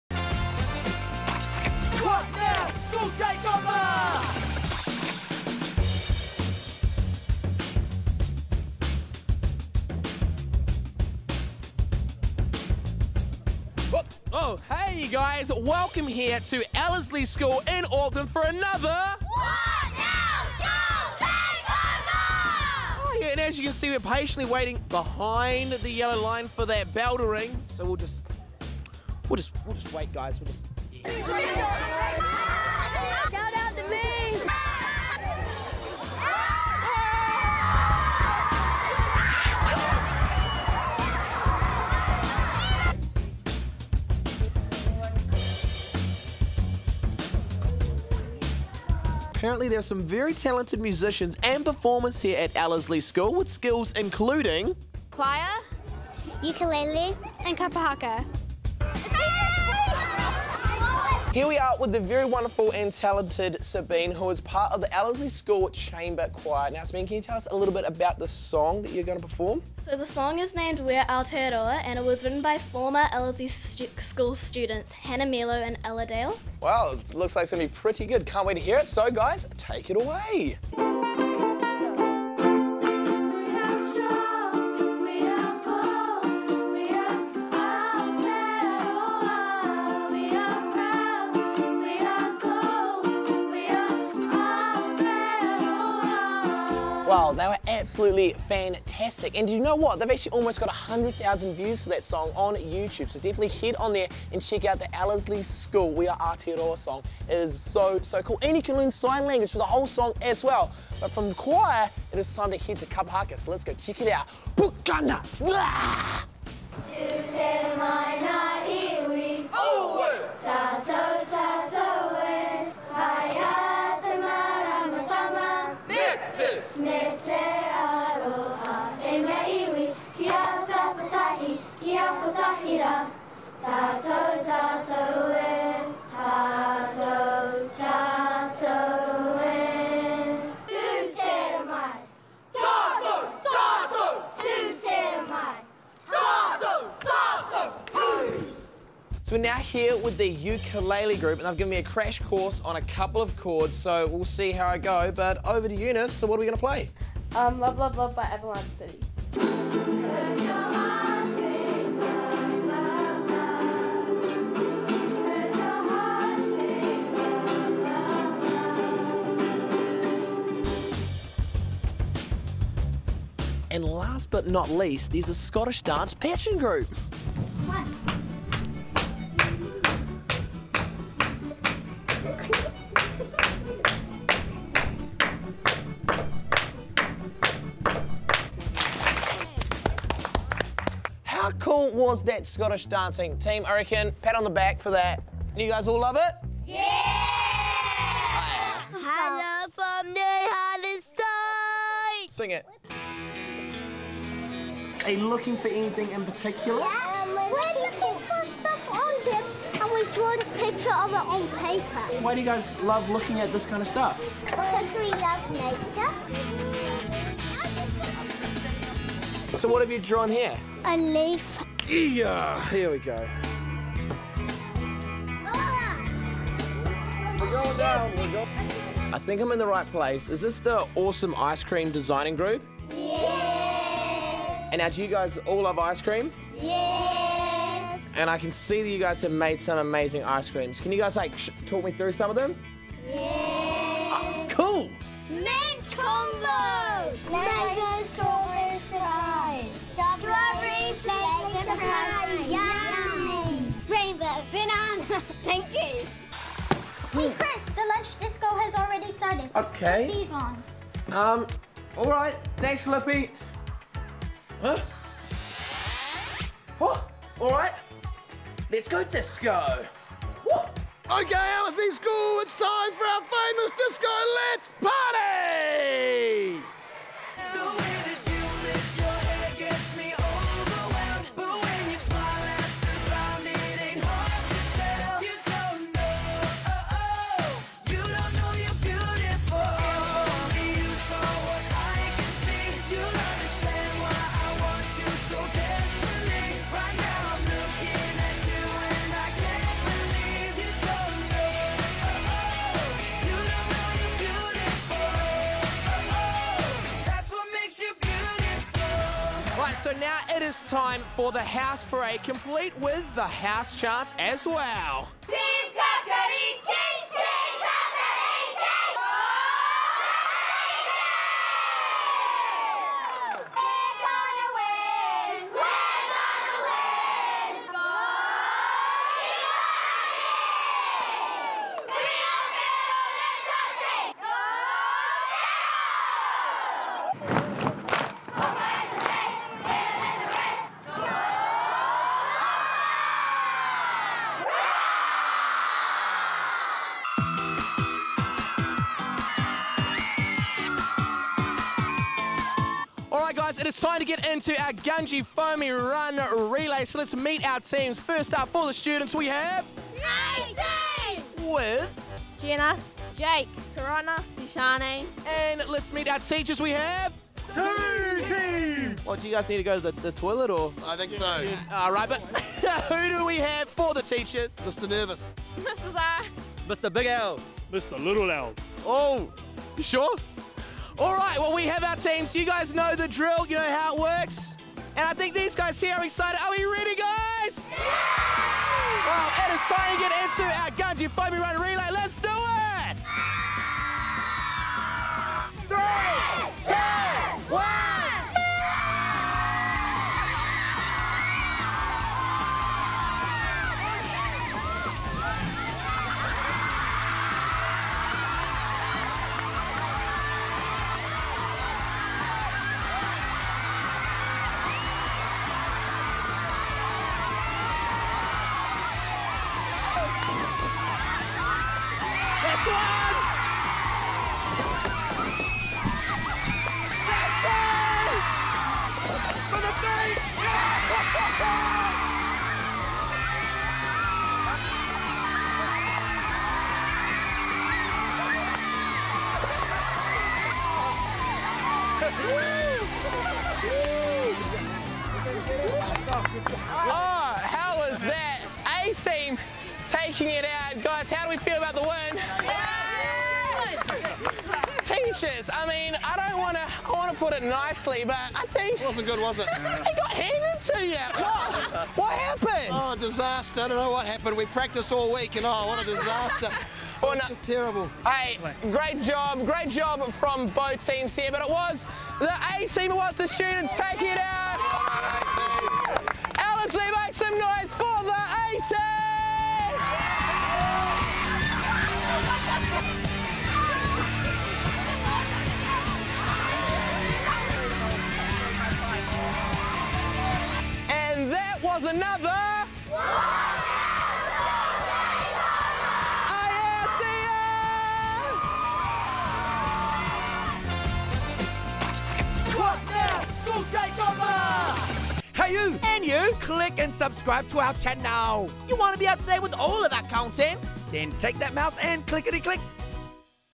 Taking Over Ellerslie School! | WN School Takeover
Chris went over to Ellerslie School and got to hang out with ALL of the incredibly awesome kids that go there!!

Made with support from New Zealand on Air!